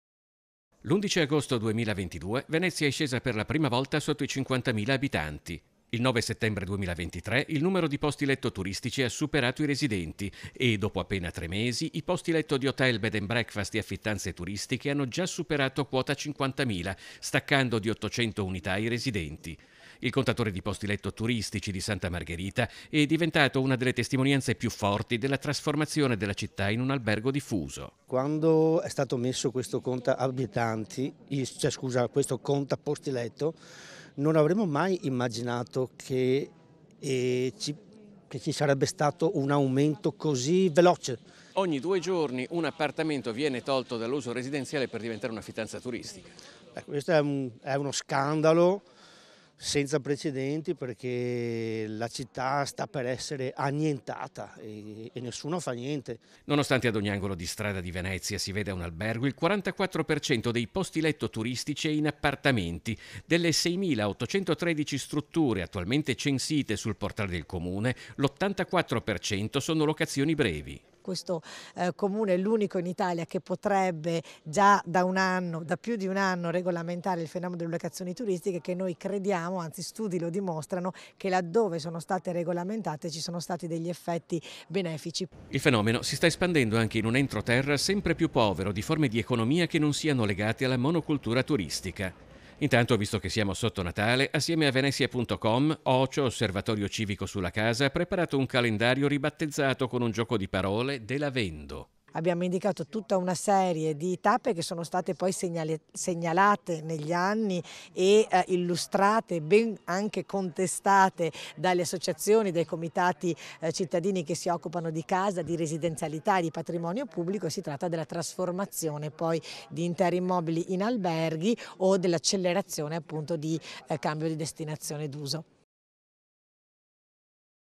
OLTRE 50.000 POSTI LETTO TURISTICI, SUPERATI ABBONDANTEMENTE I RESIDENTI | 18/12/2023
A3 NEWS Venezia 18/12/2023 - VENEZIA - Sempre più turisti, sempre meno residenti. A Venezia città è stata superata la soglia dei 50mila posti letto turistici. Il 44% sono affittanze brevi in appartamenti.  || L’11 Agosto 2022 Venezia è scesa per la prima volta sotto i 50mila abitanti. Il 9 Settembre 2023, il numero di posti letto turistici ha superato i residenti. Dopo appena tre mesi i posti letto di hotel, B&B e affittanze tursitiche hanno già superato quota 50.000, staccando di 800 unità i residenti. Il contatore di posti letto turistici di Santa Margherita è diventato una delle testimonianze più forti della trasformazione della città in un albergo diffuso.Nonostante ad ogni angolo di strada di Veneziasi veda un albergo, il 44% dei posti letto turistici è in appartamenti. Delle 6.813 strutture attualmente censite sul Portale del Comune l’84% sono locazioni brevi. Il fenomeno si sta espandendo anche in un entroterra sempre più povero di forme di economia che non siano legate alla monocultura turistica.Visto che siamo sotto Natale, assieme a venessia.com, “Ocio Osservatorio Civico sulla Casa” ha preparato un calendario ribattezzato con un gioco di parole "de la vendo". - Intervistati MATTEO SECCHI (PRESIDENTE VENESSIA.COM), MARIA FIANO (OCIO OSSERVATORIO CIVICO PER LA CASA) (Servizio di Filippo Fois) - Segui Antenna Tre anche sul digitale terrestre! Visita il sito